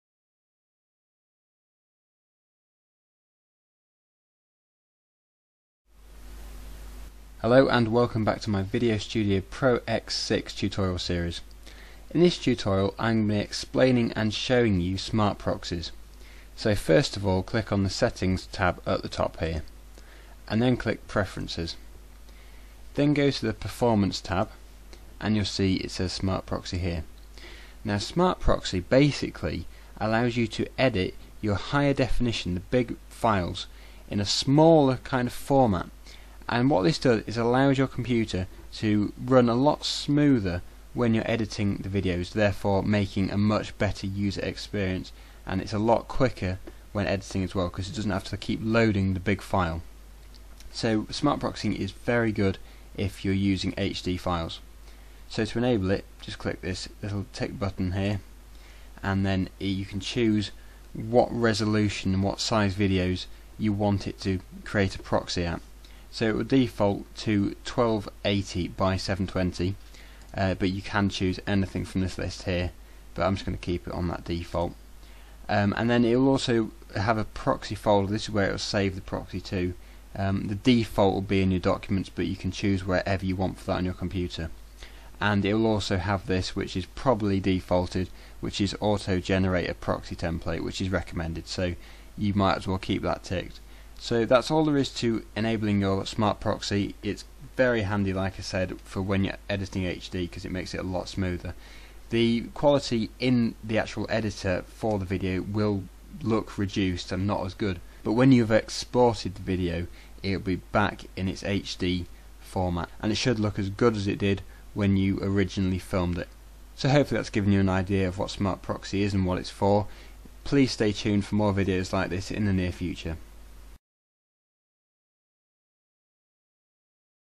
Corel VideoStudio Pro X6 - Smart Proxy Tutorial
In this Corel VideoStudio Pro X6 tutorial I show and explain what smart proxy is and how to use it. Smart proxy is a very useful function when editing your videos.